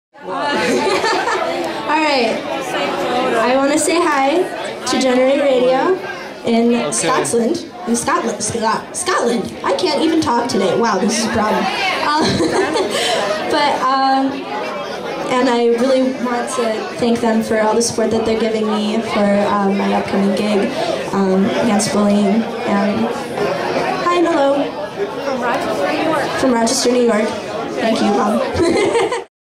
Stand up and stand out!-shout out! to Generate Radio for support!
Stand up & Stand out! Shout out from Tivoli Skye @ Lovin Cup in Roch. NY USA to Berwickshire Scotlands Donald Strachan @ Generate Radio !